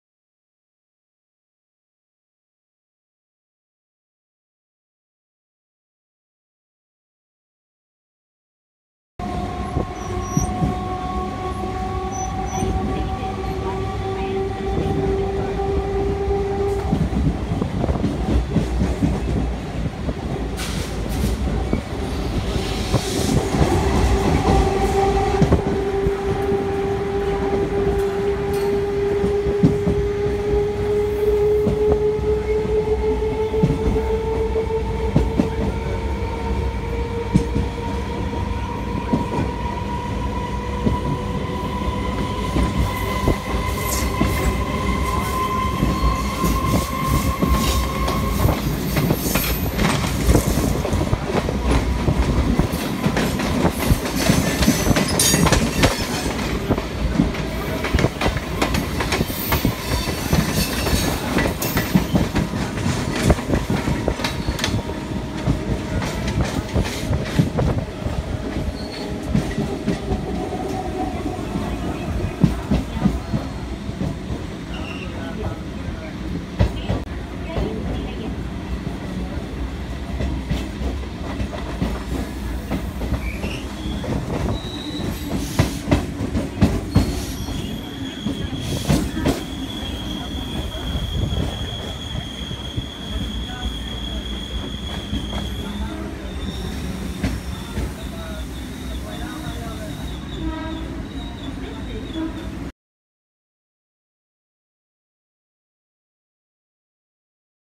From MAS TO BBQ
From MAS TO BBQ dated  9.5.18 captured by me on board mas-avd local